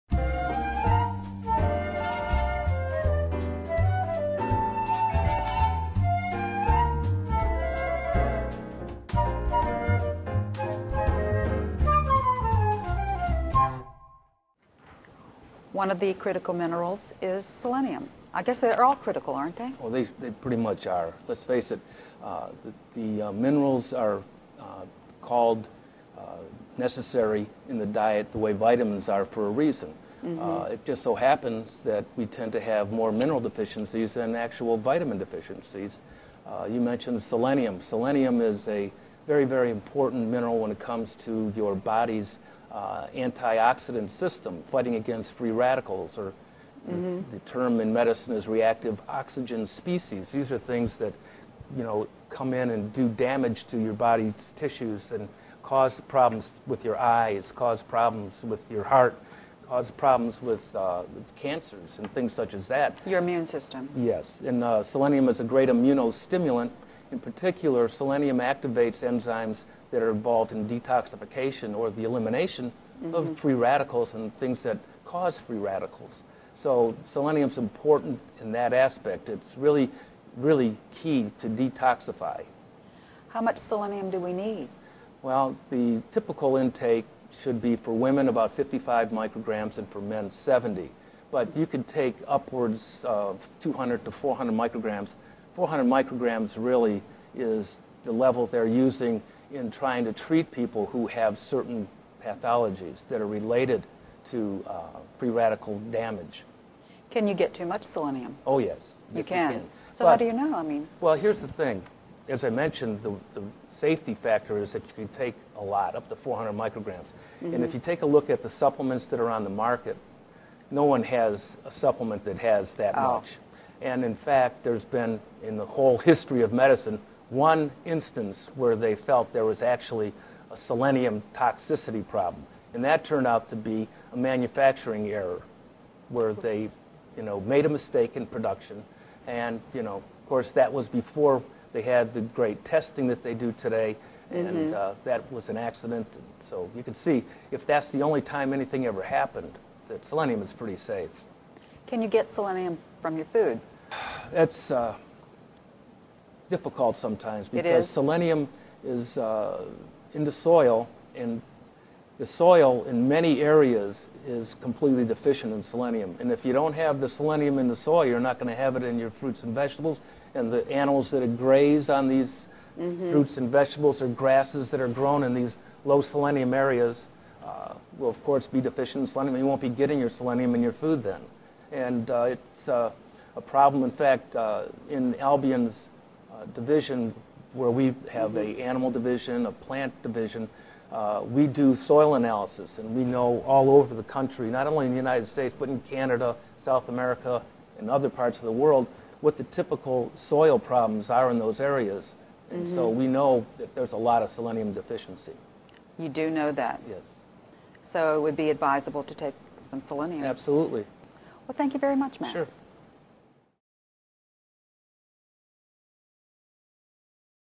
Selenium
Minerals are necessary in your diet. Pharmacist Max Motyka looks at why selenium is such an important mineral because of its antioxidant value.